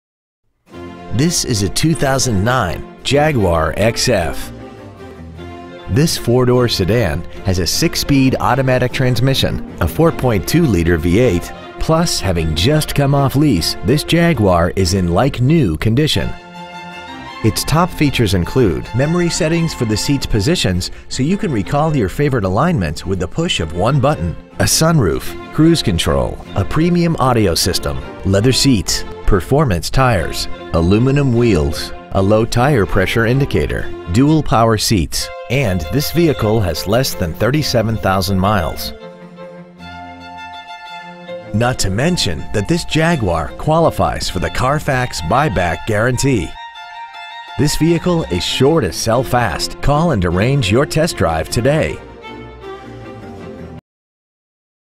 2009 Jaguar XF Ft. Lauderdale FL
Year : 2009

 Make : Jaguar

 Model : XF

 Engine : Gas V8 4.2L/256

 Trans . : Automatic

 Exterior : Frost Blue

 Miles : 36,338

 Interior : Dove/charcoal

 Stock : 33060

 Offleaseonly.com

 3531 Lake Worth Rd

 2009 Jaguar XF Lake Worth FL